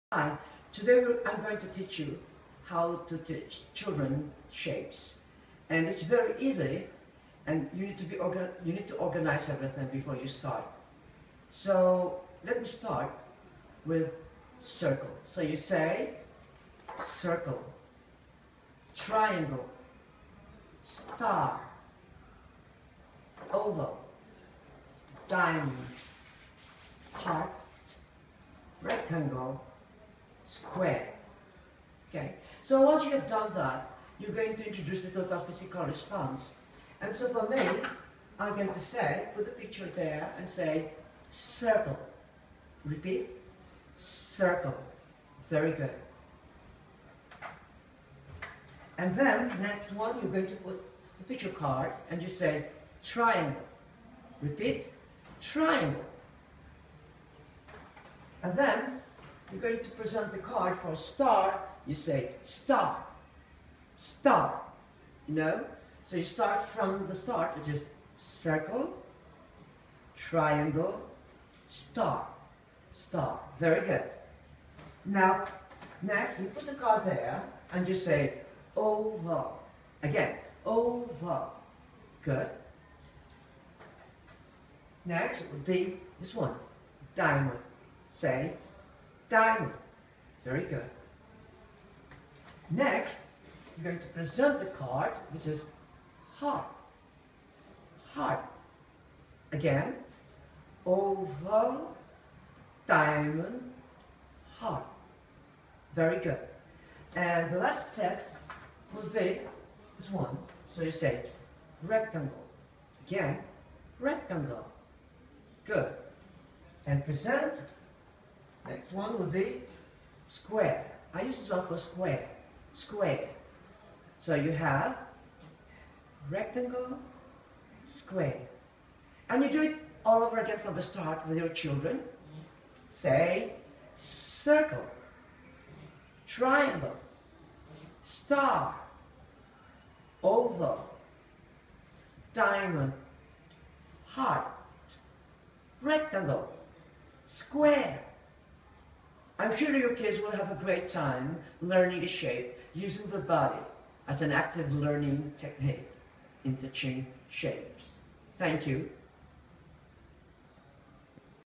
How to teach young learner Shapes
Effective way of giving presentation on shapes in less than three minutes.